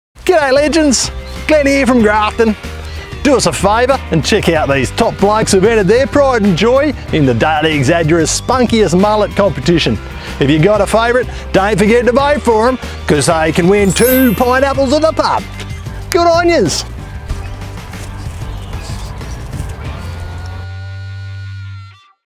Spunkiest Mullet (III)
Bogan character Glenn from Grafton promoting The Daily Examiner's Spunkiest Mullet Competition.